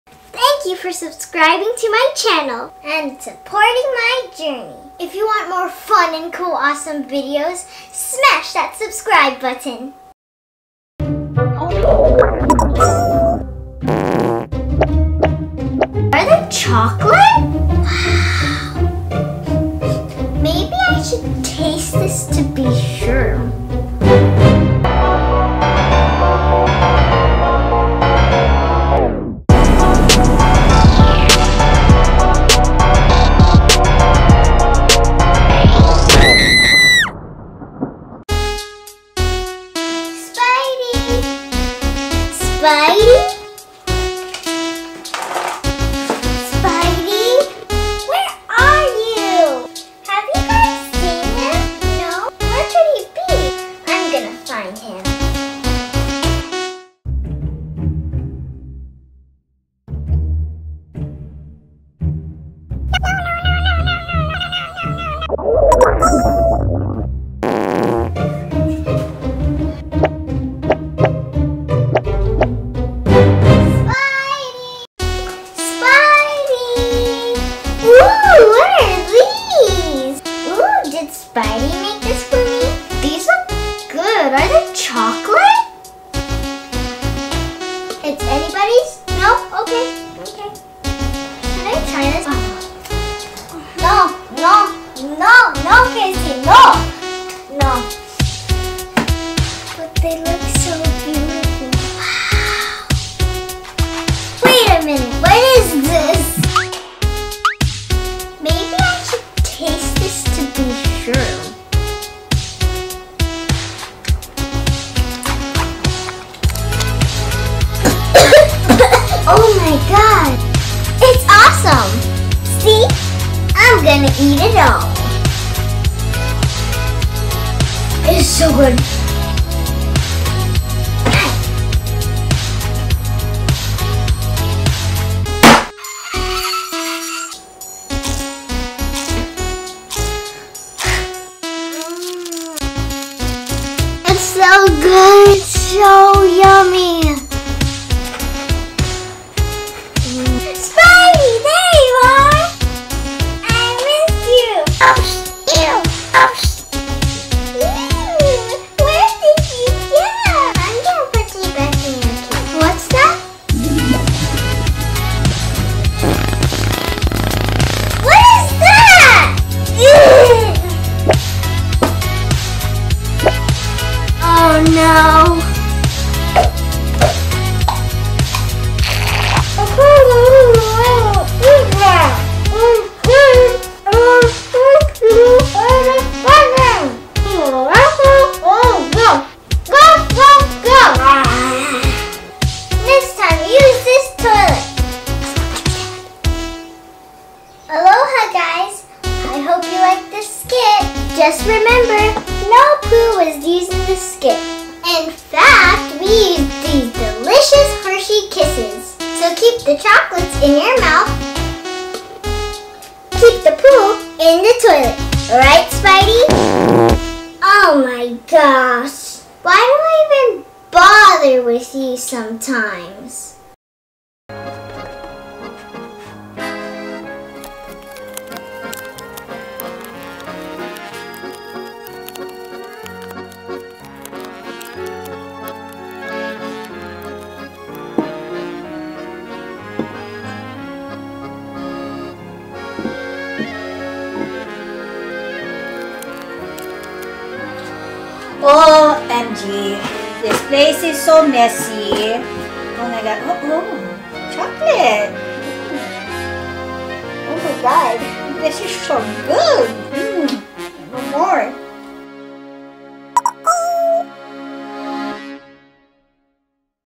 Chocolate Poop Prank Comedy Skit!
In this chocolate poop prank comedy skit, Spidey leaves a trail of chocolate poop which KayCee accidentally eats. Watch how it unravels in this episode of KayCee's Corner. There is a secret ending to this episode.

Episode 4 Chapters
0:00 - Intro
0:11 - Intro Sequence
0:40 - Kaycee looks for Spidey
0:57 - Spidey needs to use the restroom
1:18 - Kaycee stumbles on chocolate treats
1:50 - Kaycee devours chocolate poo
2:31 - Spidey is discovered
2:43 - Kaycee finds out what she really ate
2:56 - Kaycee is disgusted and brushes her teeth
3:17 - What we used for chocolate poo
3:44 - Secret Ending